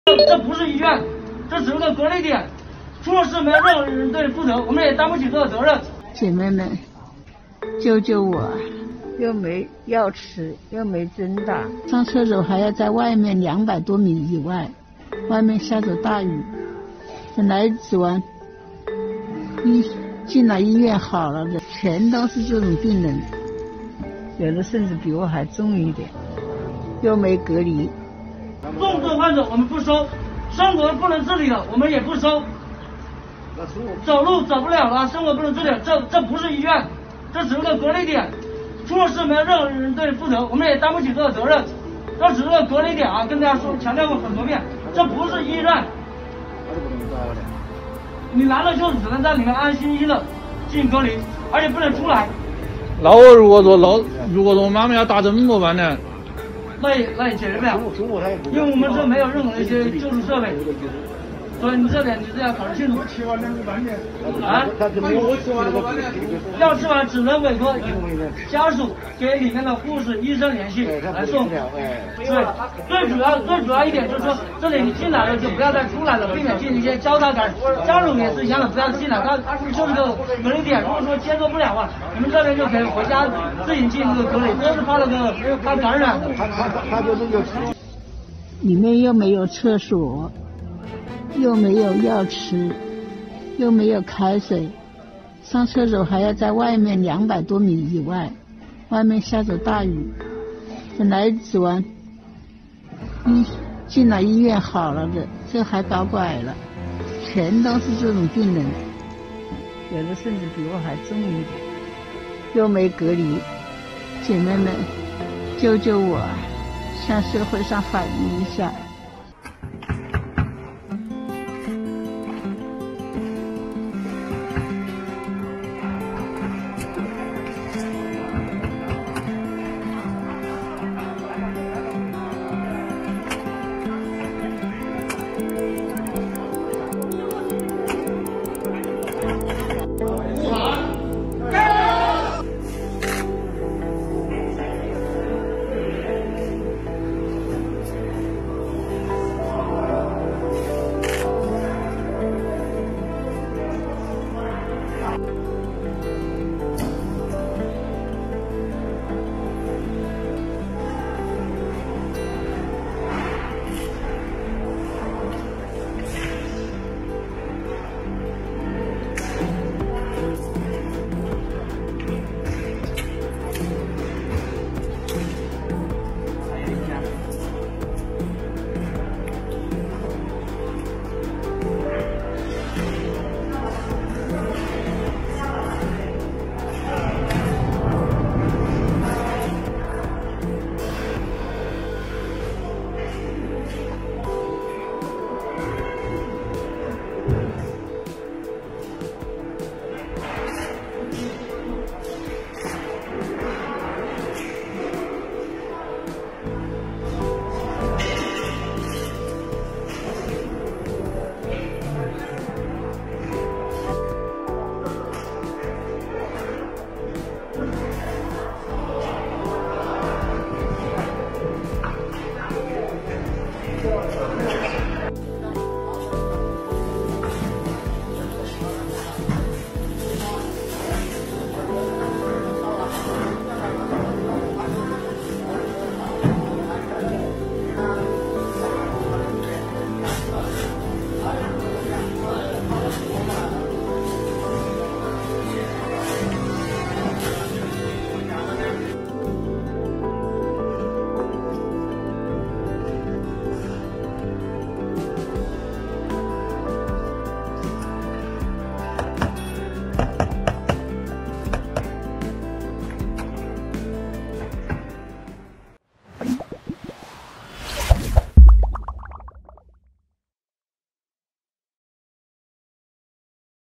病患錄影曝方艙醫院大通鋪 負責人：重症病人我們不收
追蹤【中時電子報IG】！掌握藝人們最新動態